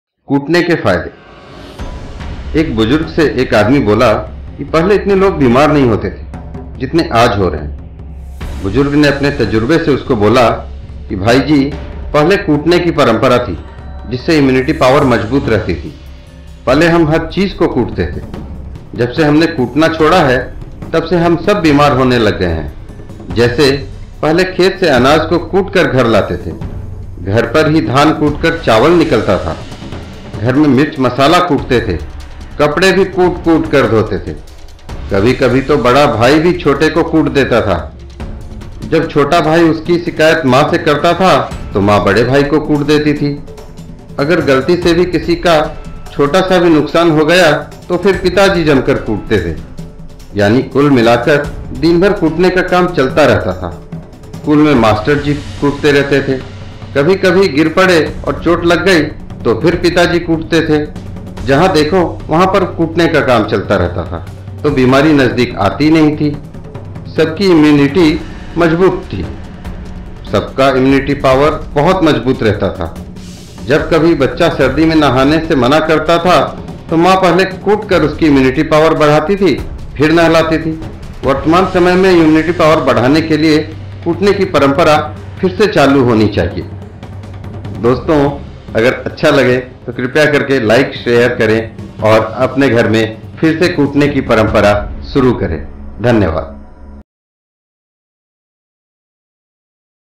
कूटने के फायदे....
वर्तमान समय में इम्यूनिटी पावर बढ़ाने के लिए कूटने की परंपरा फिर से चालू होनी चाहिए...